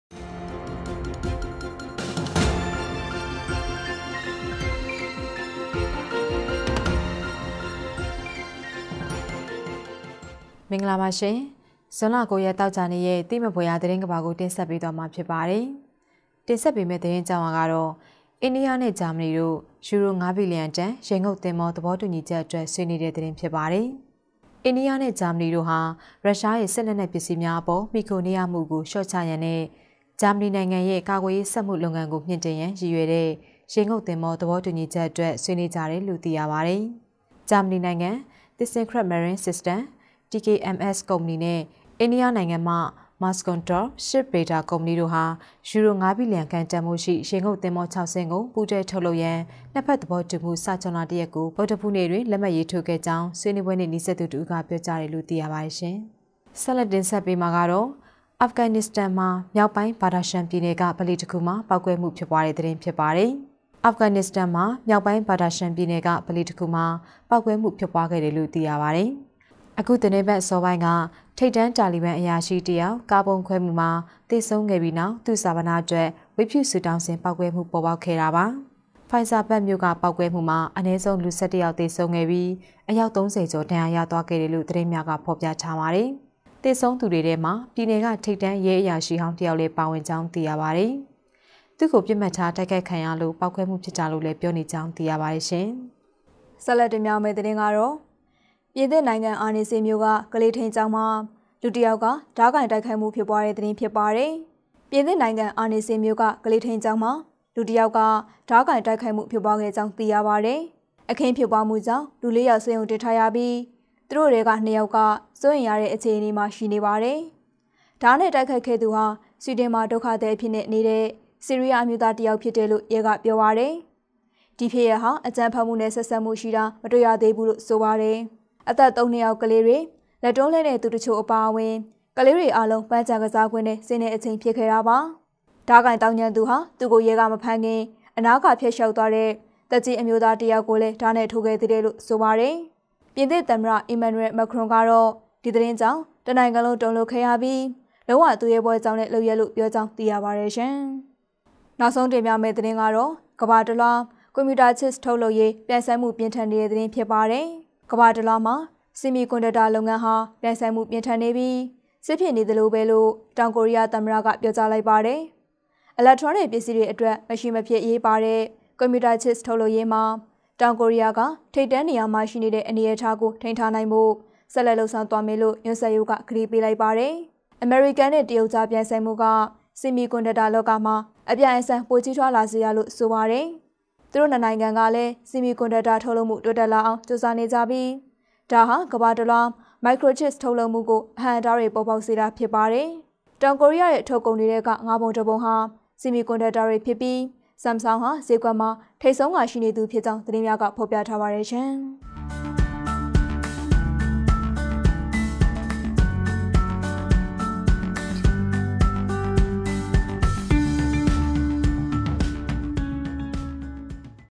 သိမှတ်ဖွယ်ရာ သတင်းကမ္ဘာ (၉.၆.၂၀၂၃)
သိမှတ်ဖွယ်ရာသတင်းကမ္ဘာ
(၉.၆.၂၀၂၃)
- အိန္ဒိယနှင့် ဂျာမနီတို့ ယူရို (၅) ဘီလီယံတန် ရေငုပ်သင်္ဘော သဘောတူညီချက်အတွက် ဆွေးနွေး
- အာဖဂန်နစ္စတန်မှာ မြောက်ပိုင်း ဘါ့ခ်ဒါရှန်ပြည်နယ်က ဗလီတစ်ခုမှာ ပေါက်ကွဲမှုဖြစ်ပွား
- ပြင်သစ်နိုင်ငံ အာနေဆေးမြို့က ကလေးထိန်းကျောင်းမှာ လူတစ်ယောက်မှ ဓားကိုင်တိုက်ခိုက်မှု ဖြစ်ပွား
- ကမ္ဘာတလွှား ကွန်ပြူတာချစ်ပ် ထုတ်လုပ်ရေး ပြိုင်ဆိုင်မှုပြင်းထန်နေ